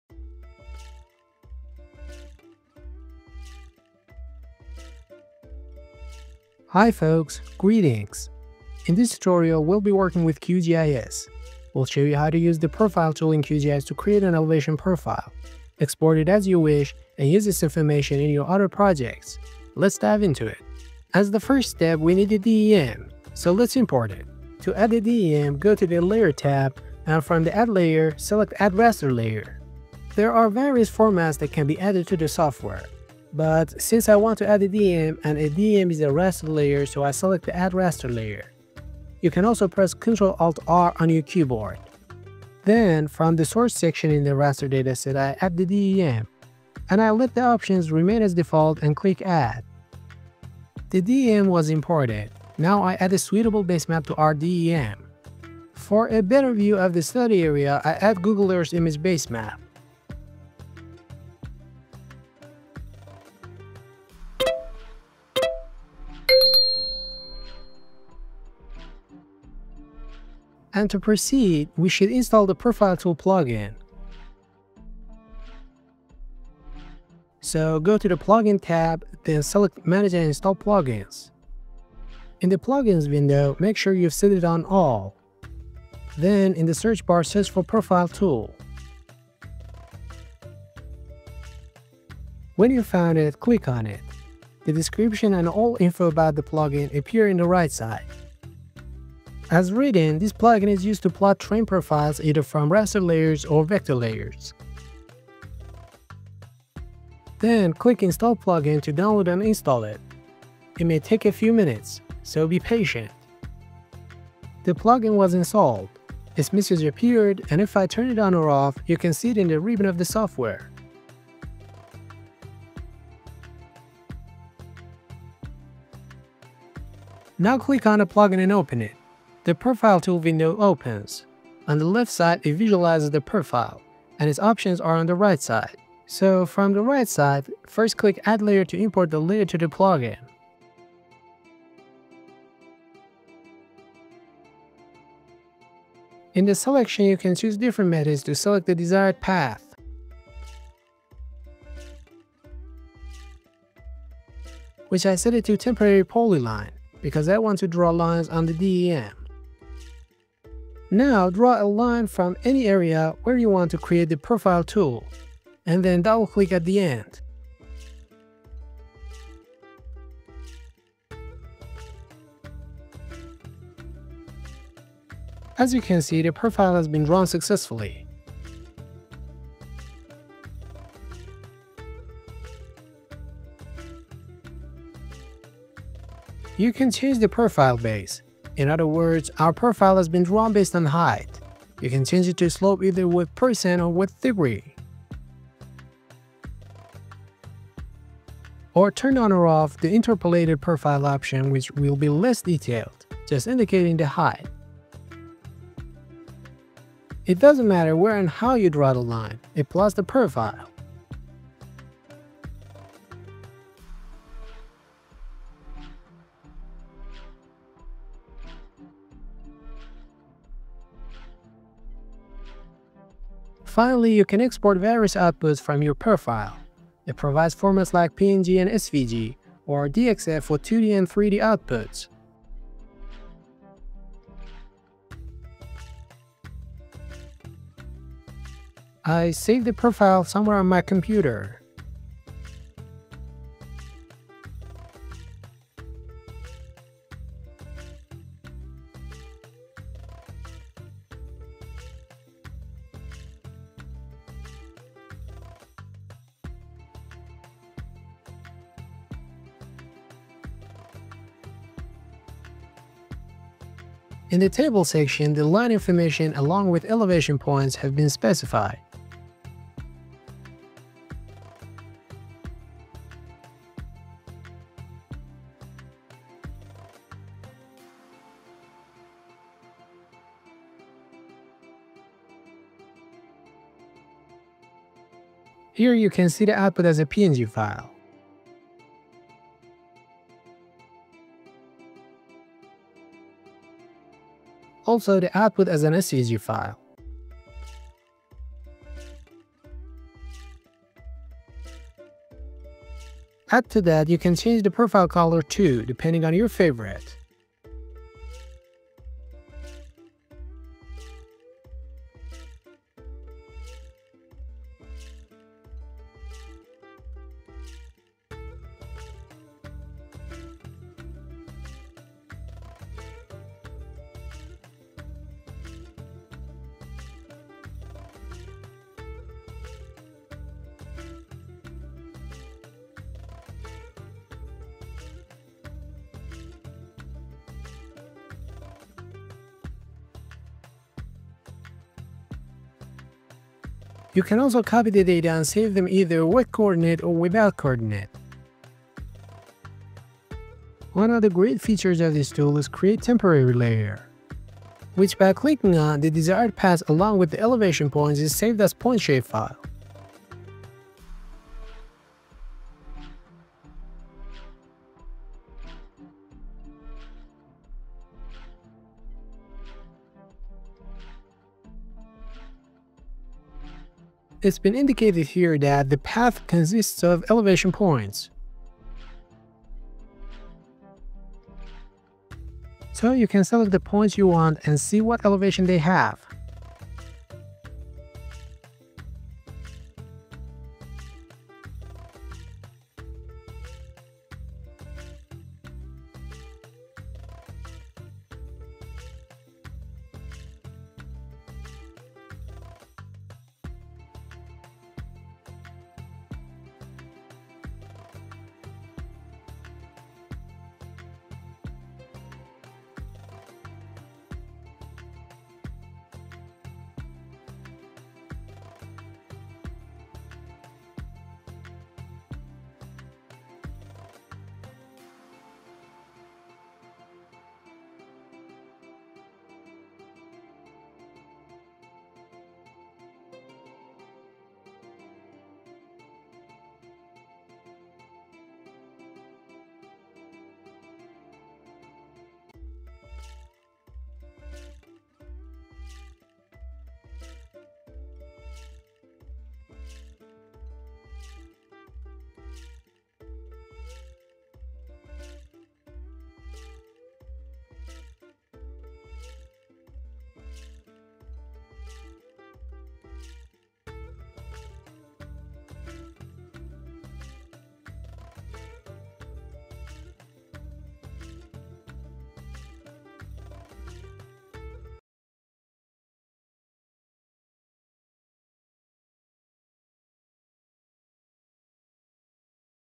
How to Plots terrain profile in QGIS using Profile tool Plugin
In this video, you'll learn how to use the Profile Tool in QGIS to create elevation profiles. Follow along step by step to discover how to import DEM layers, select paths, and create precise elevation profiles. This tutorial is perfect for those interested in spatial and geographic analysis.

Let’s dive into the world of GIS together! 🌍